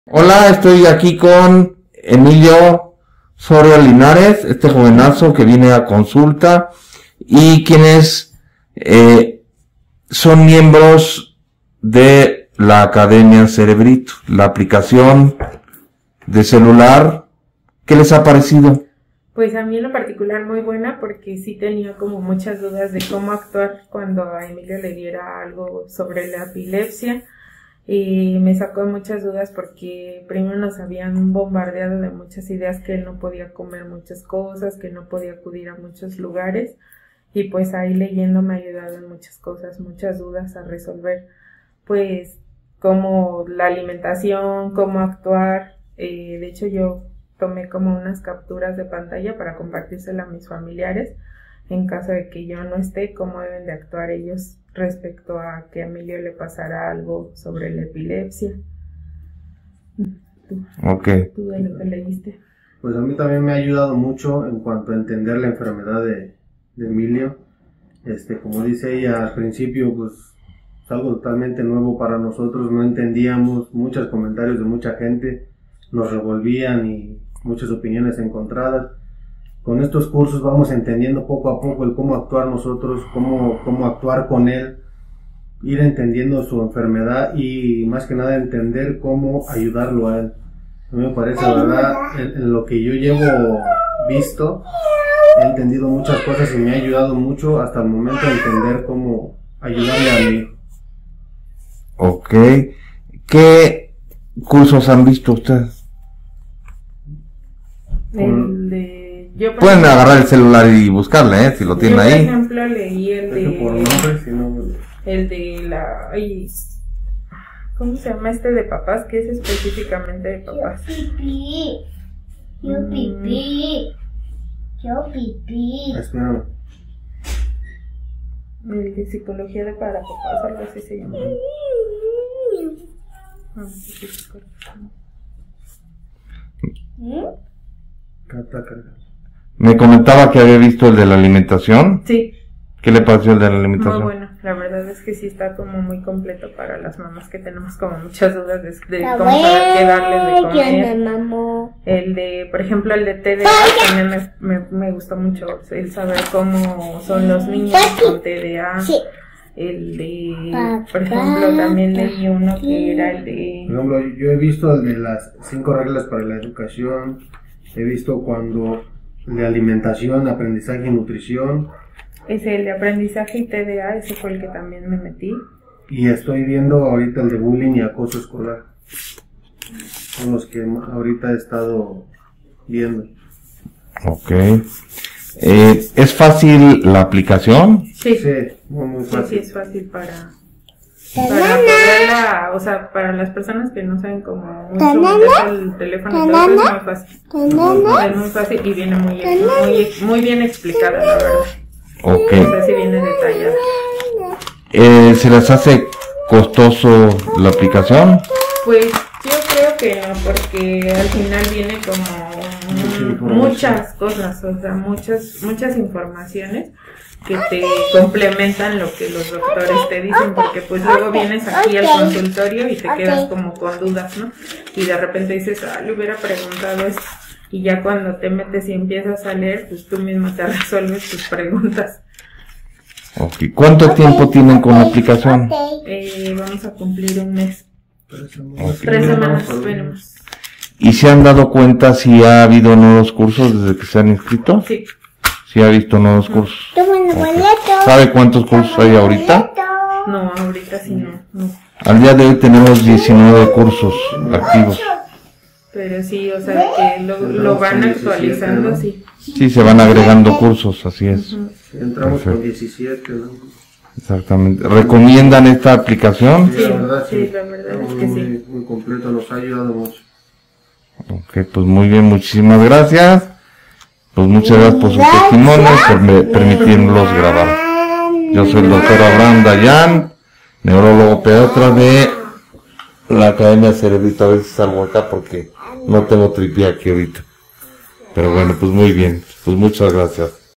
Testimonio Emilio Soria Linares 01ago2023
Emilio Soria Linares Nos platica su testimonio siendo Miembro en la Academia Cerebrito

Descarga la App de la mejor Escuela para Padres, La Academia Cerebrito